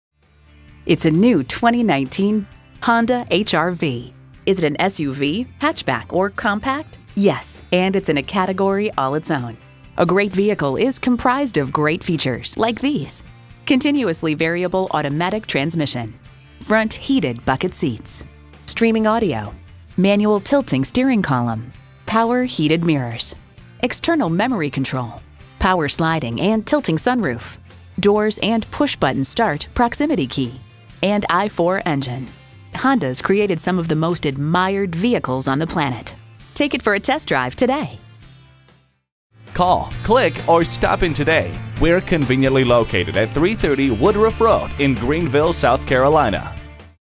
New 2019 Honda HR-V Greenville SC Easley, SC #190823 - SOLD
Year: 2019
Make: Honda
Model: HR-V
Trim: EX
Engine: 1.8L I4 SOHC 16V i-VTEC
Transmission: CVT
Color: Red
Interior: Blk Clth
Stock #: 190823
VIN: 3CZRU5H55KM705864

Address:
330 Woodruff Road